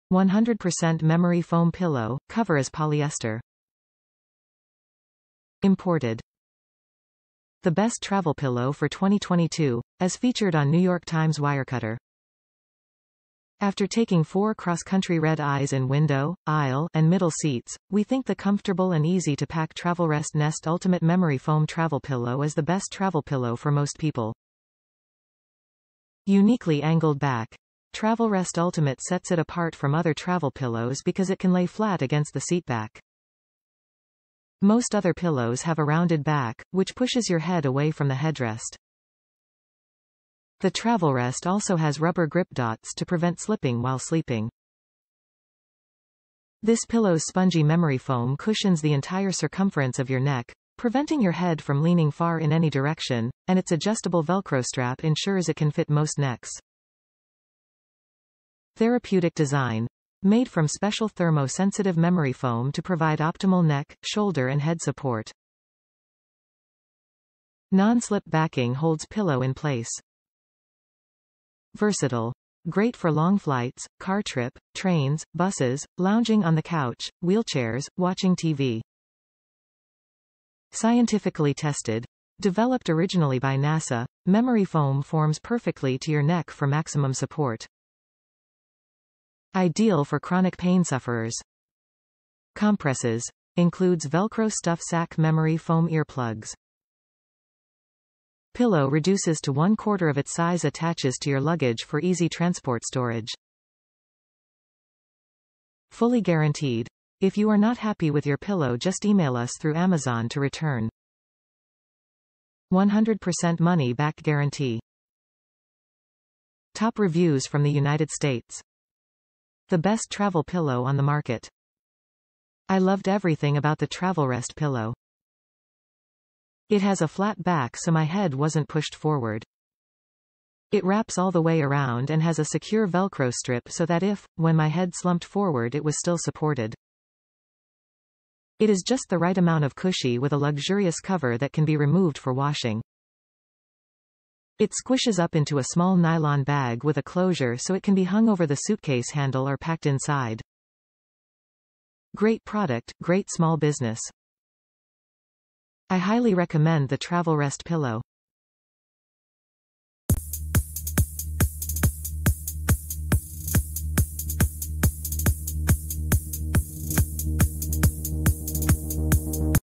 Travelrest Nest Patented Memory Foam Travel Pillow / Neck Pillow - Washable
100% Memory Foam Pillow, Cover is Polyester
Imported
THE BEST TRAVEL PILLOW FOR 2022: As featured on New York Times Wirecutter. After taking four cross-country red-eyes in window, aisle, and middle seats, we think the comfortable and easy-to-pack Travelrest Nest Ultimate Memory Foam Travel Pillow is the best travel pillow for most people.
UNIQUELY ANGLED BACK: Travelrest Ultimate sets it apart from other travel pillows because it can lay flat against the seat back. Most other pillows have a rounded back, which pushes your head away from the headrest. The Travelrest also has rubber grip dots to prevent slipping while sleeping. This pillow’s spongy memory foam cushions the entire circumference of your neck, preventing your head from leaning far in any direction, and its adjustable Velcro strap ensures it can fit most necks.
THERAPEUTIC DESIGN: Made from special thermo sensitive memory foam to provide optimal neck, shoulder and head support. Non-slip backing holds pillow in place.
VERSATILE: Great for long flights, car trip, trains, buses, lounging on the couch, wheelchairs, watching TV.
SCIENTIFICALLY TESTED: Developed originally by NASA, memory foam forms perfectly to your neck for maximum support. Ideal for chronic pain sufferers.
COMPRESSES: Includes velcro stuff sack & memory foam ear plugs. Pillow reduces to 1/4 of it's size & attaches to your luggage for easy transport & storage.

 The best travel pillow on the market.
I loved everything about the Travelrest pillow. It has a flat back so my head wasn't pushed forward. It wraps all the way around and has a secure velcro strip so that if/when my head slumped forward it was still supported. It is just the right amount of cushy with a luxurious cover that can be removed for washing. It squishes up into a small nylon bag with a closure so it can be hung over the suitcase handle or packed inside. Great product, great small business. I highly recommend the Travelrest pillow!